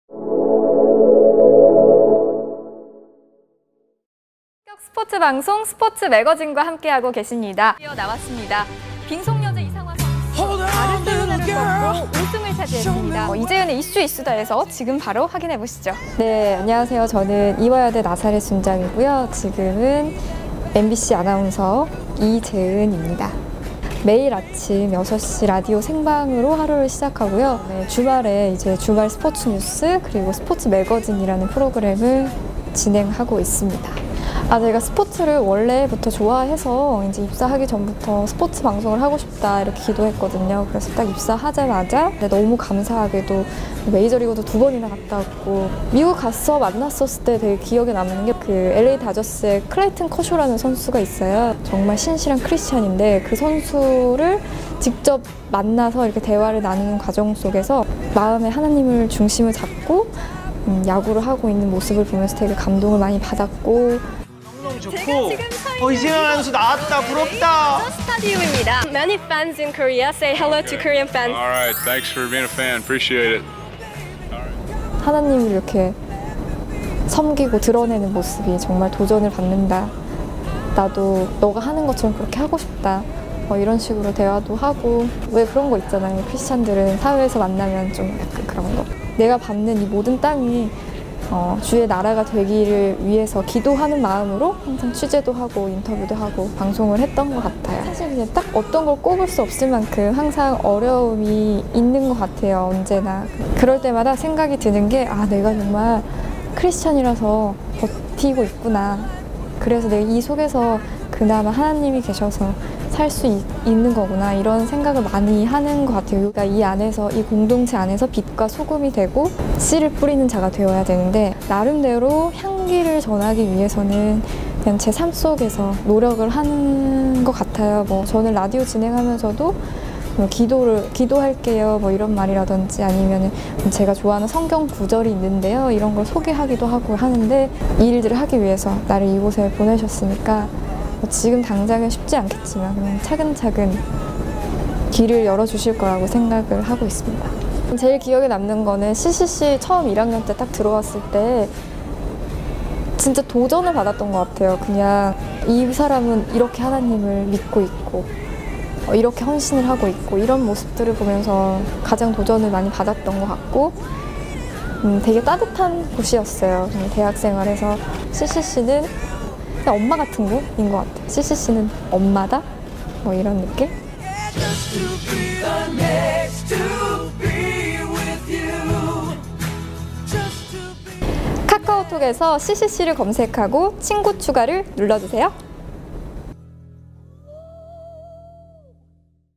[CCC] My Best Experience 1 - 이재은 아나운서
이재은 MBC아나운서는 이화여대 CCC(서울 서지구)출신으로
사회적으로나 신앙적으로 많은 사람들의 귀감이 되고 있습니다.

★본 영상을 활용한 소그룹 모임(온/오프라인) 자료들이 아래 웹사이트에 있습니다. (무료)
 ccclab.kr

웹사이트에서 더욱 다양한 영상과 그에 따른 활용 자료를 다운로드하세요!